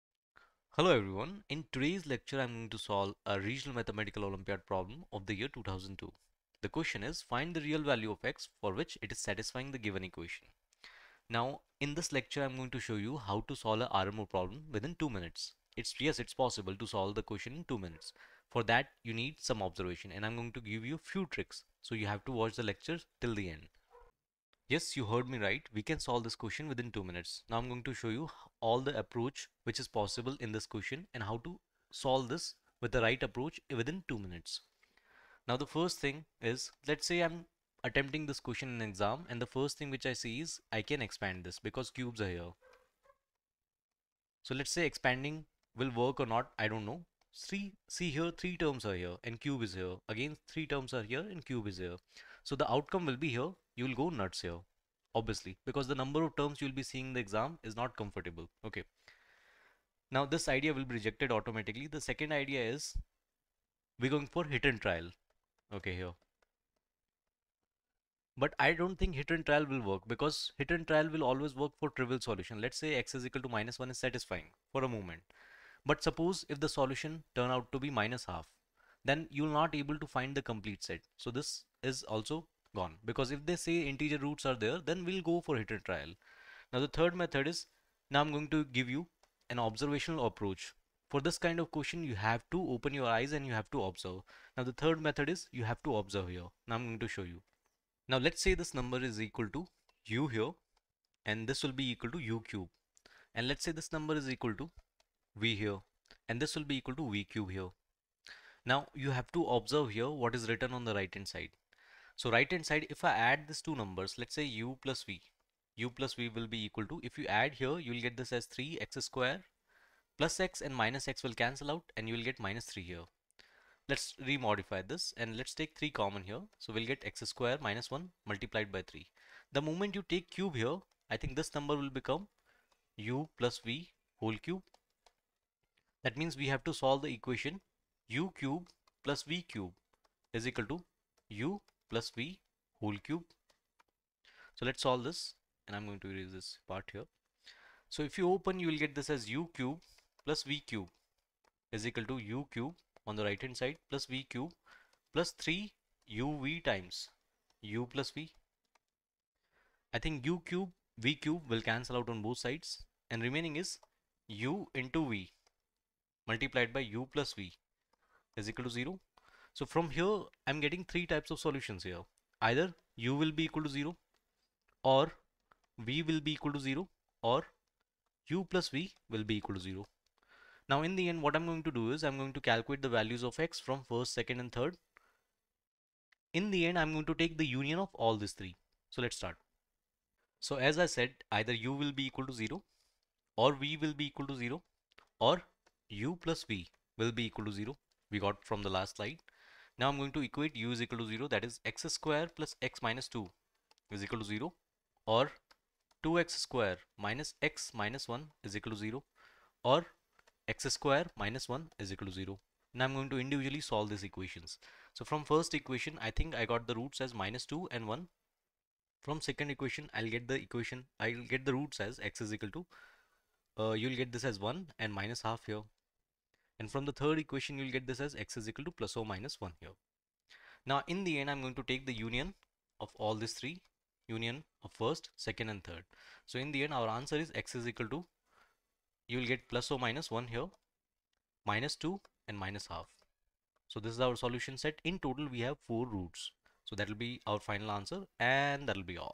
Indian Mathematical Olympiad || RMO 2002
We present a solution to question from the 2002 Indian Mathematical Olympiad | RMO. The goal of this question is to find all the Real values of a for which the given equation has real roots in x.